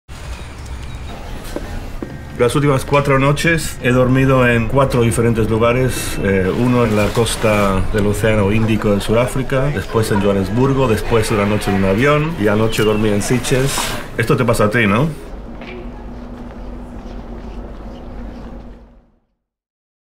A prop. Una conversa privada entre Rada Nadal & John Carlin - BANC SABADELL
A prop és un projecte de Banc Sabadell. Una conversa privada entre Rafa Nadal & John Carlin que, com la majoria de converses d'avui dia, no passa en un espai i temps concrets, sino en molts. Londres, Manacor, Pequín, Sitges, Johannesburg o a qualsevol on les seves vides els portin. Perquè tampoc té lloc a través d'un sol mitjà, sinó a través de tots: missatgeria instantània, videoconferència, correu electrònic i trucades telefòniques.